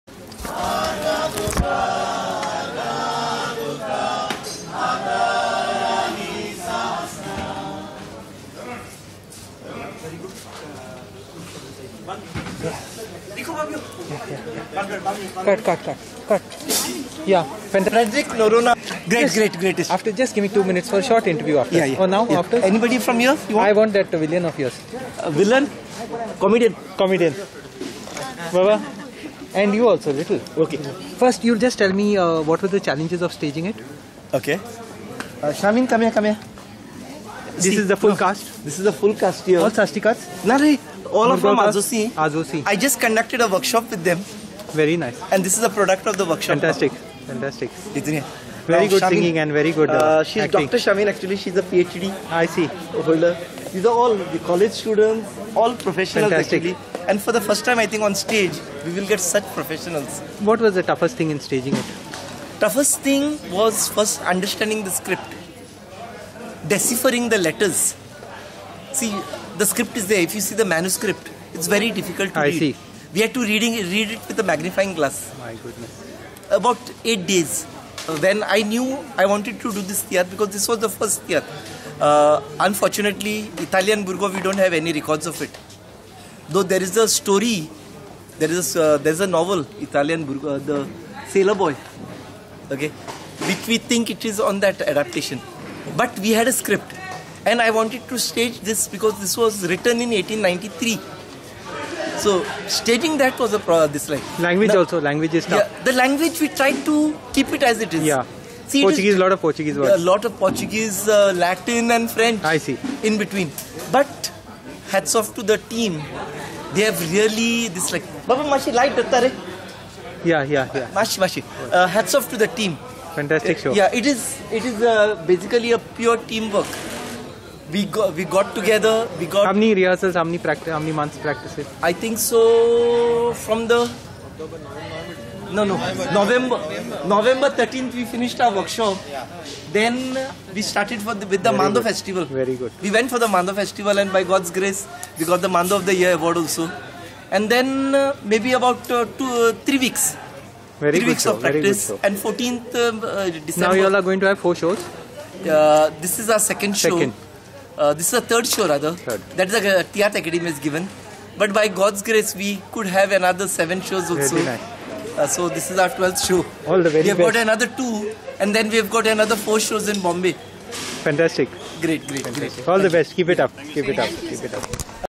Reviving a 130-year-old Konkani play, The Belle of Cavelle, Cavelachi Sundori
By MNichael Gracias and troup, an old tiatr with a new lease of life. When I walked backstage after the show, I found them encircled in a thanksgiving prayer! Here's an informal chat with Michael and team... very brief at almost the midnight hour after a three-long performance of music and drama-filled theatre in the Konkani language. Tiatr is a form long neglected but which has come to be appreciated for its strengths and uniqueness (including by scholars such as Prof Pramod Kale, and Prof A Rafael Fernandes). Recorded by Frederick Noronha (FN).